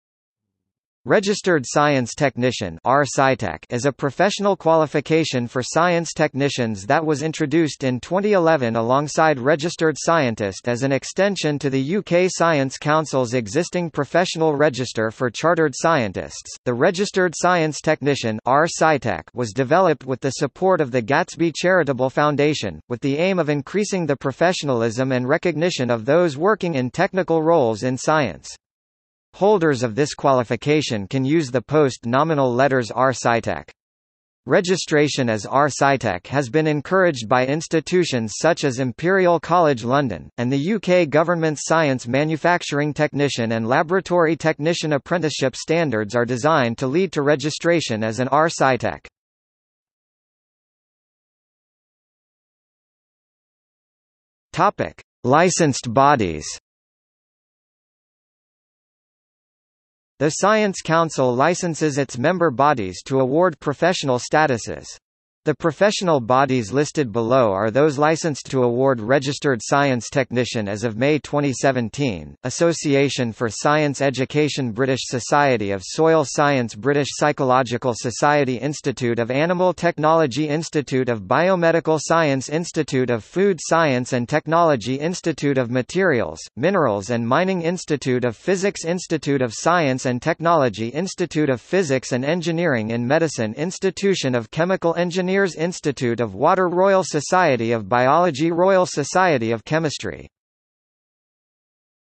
Registered Science Technician | Wikipedia audio article
00:00:51 Licensed Bodies

- Socrates

SUMMARY
Registered Science Technician (RSciTech) is a professional qualification for science technicians that was introduced in 2011 alongside Registered Scientist as an extension to the UK Science Council's existing professional register for Chartered Scientists. the Registered Science Technician (RSciTech) was developed with the support of the Gatsby Charitable Foundation, with the aim of increasing the professionalism and recognition of those working in technical roles in science. Holders of this qualification can use the post-nominal letters RSciTech. Registration as RSciTech has been encouraged by institutions such as Imperial College London, and the UK Government's Science manufacturing technician and Laboratory technician apprenticeship standards are designed to lead to registration as an RSciTech.